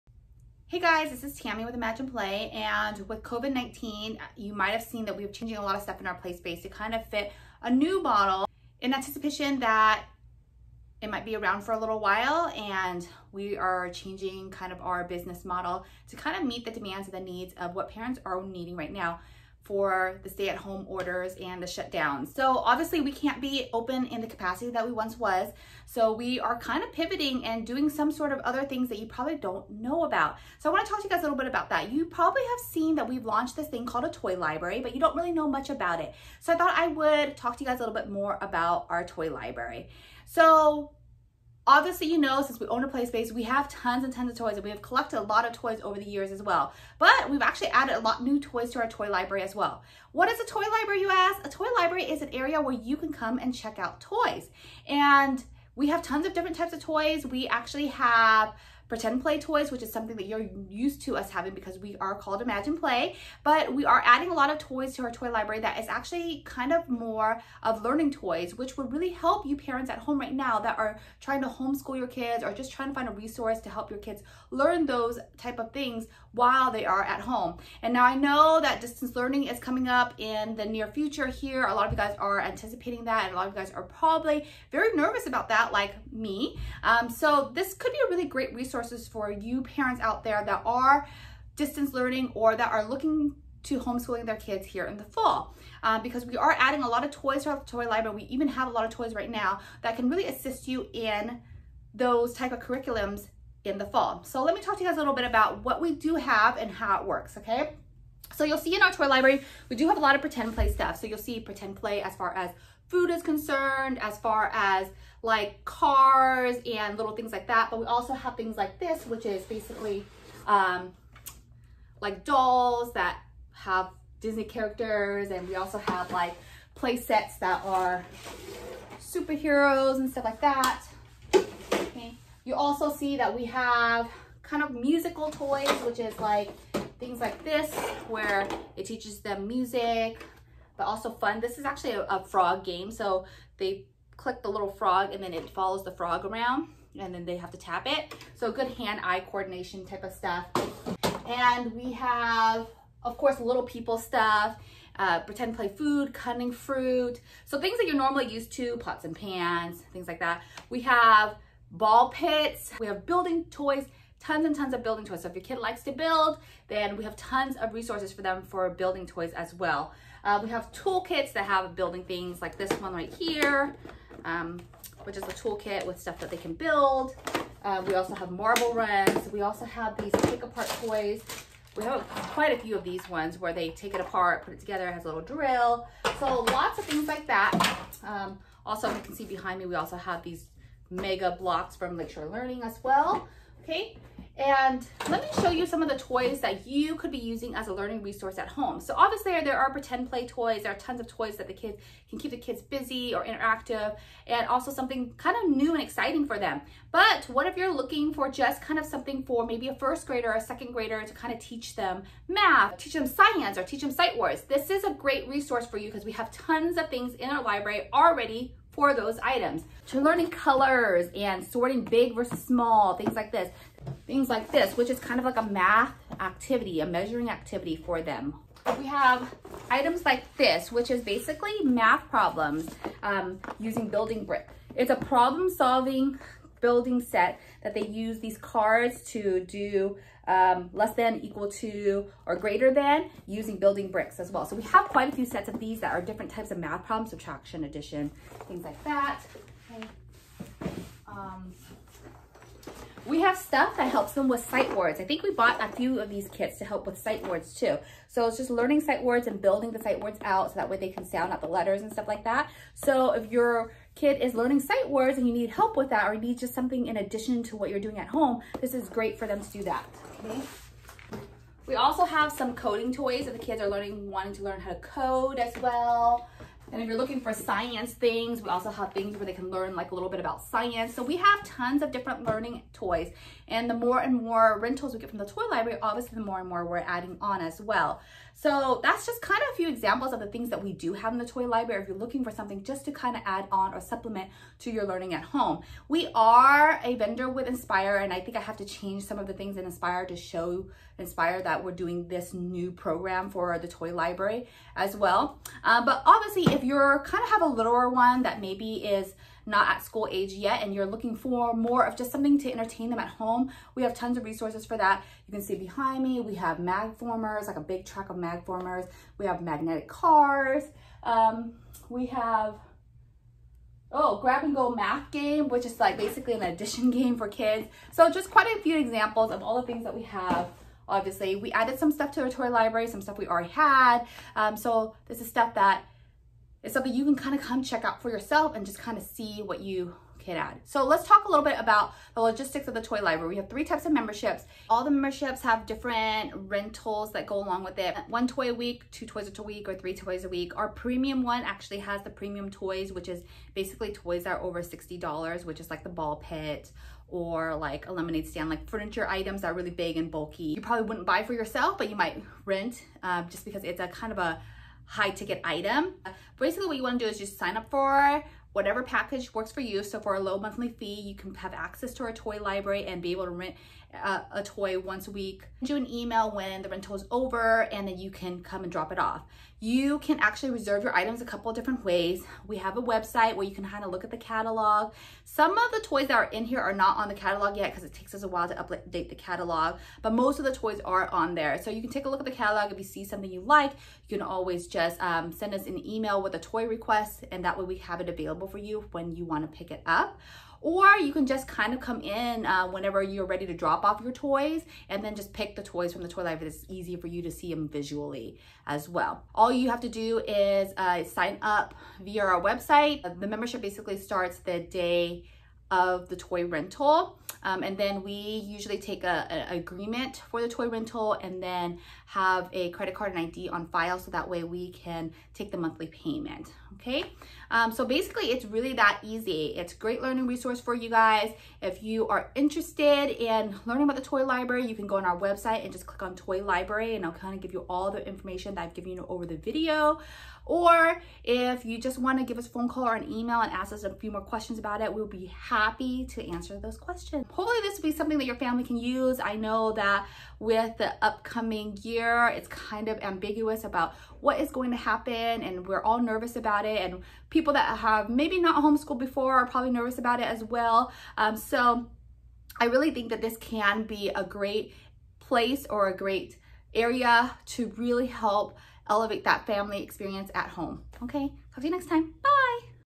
What's in our toy library?
In this video we share more info about our toy library and how it can be a learning resource for you at home. We give you example of the toys we have and other learning toys for the fall school curriculum. With anticipation of more homeschooling and distant learning see what we we have to help your child at home.